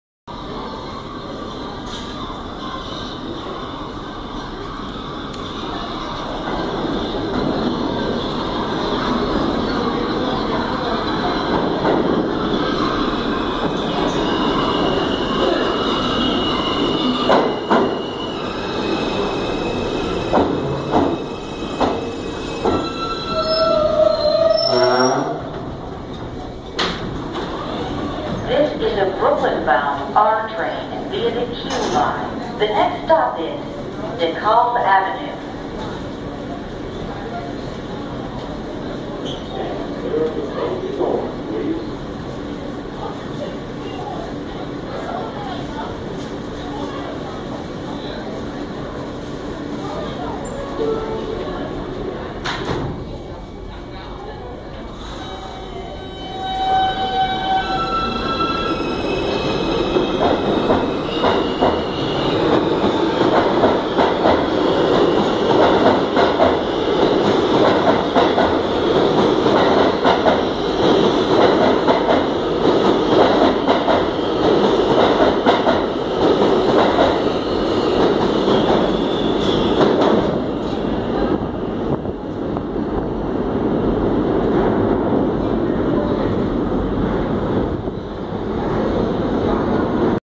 95th Street - Bay Ridge Bound Train Of R160As & R160Bs On The R @ Canal Street
Seen here is a train of R160s operating on the R to Bay Ridge arriving and departing from Canal Street Station.